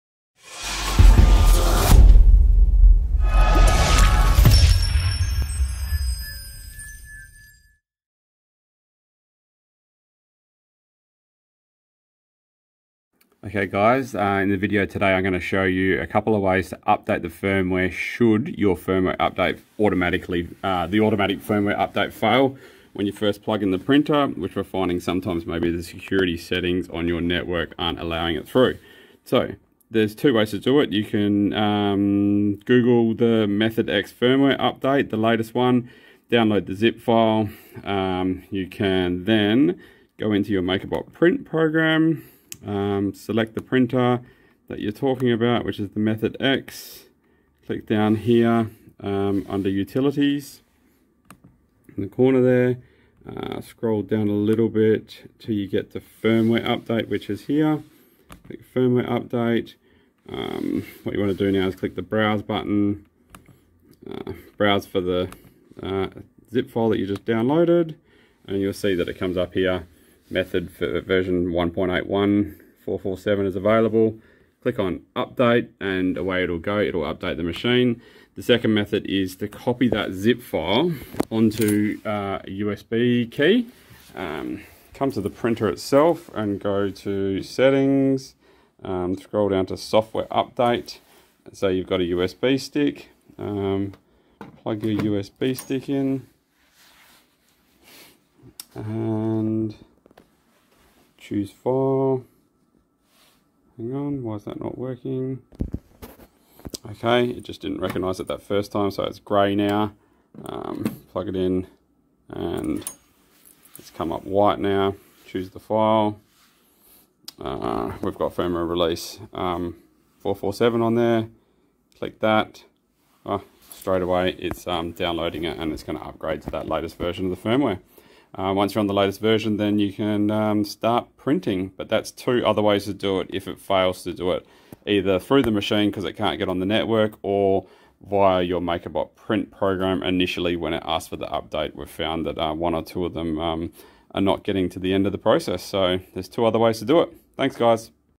How to update the firmware on your Makerbot Method using USB or USB Stick
The video will show you 2 other firmware update options for your Makerbot Method printer when your printer wont auto update due to restrictions on your internet, mostly due to highly secure education networks.  I will show you how to manually download the update and force via Makerbot Print program, or to update via USB stick on the printer.